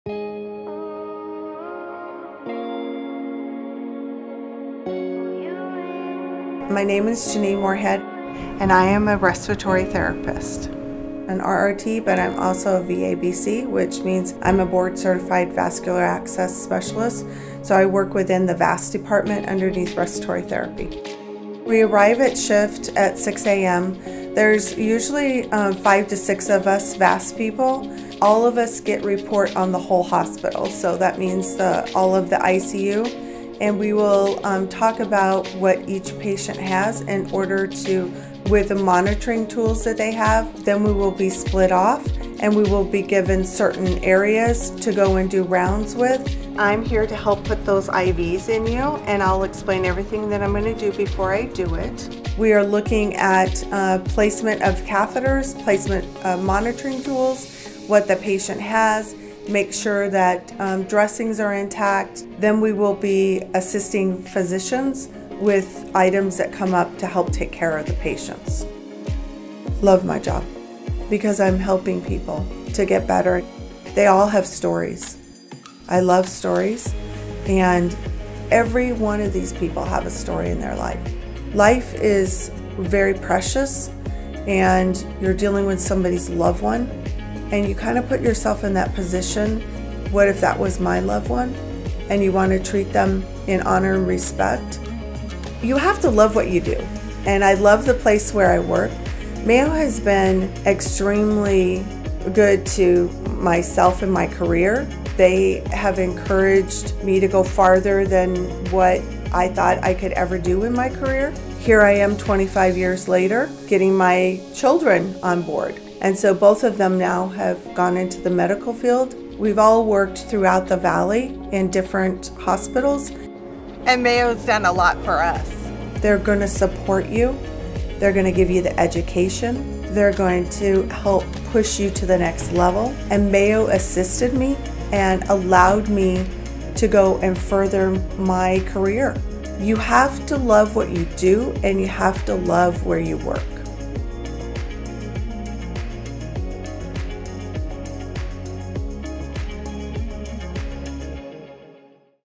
Respiratory Therapist
This video describes the respiratory therapist role in patient care from placing IVs, to catheters, to monitoring tools, and assisting physicians. It also describes the many career opportunities there are within healthcare at a large medical center. It demonstrates the importance of teamwork, and specifically an eye for detail.